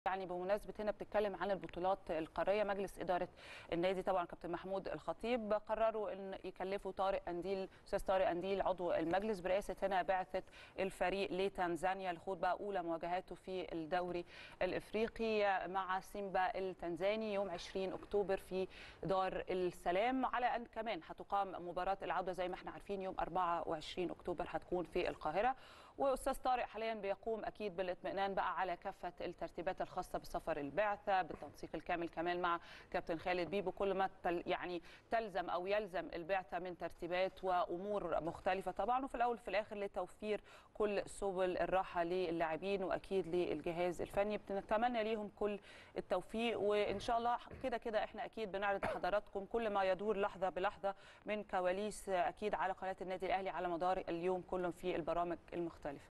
نهاوند سري: طارق قنديل رئيسًا لبعثة الأهلي في تنزانيا لخوض أولى مواجهاته في الدوري الإفريقي
نهاوند سري: طارق_قنديل رئيسًا لبعثة الأهلي في تنزانيا لخوض أولى مواجهاته في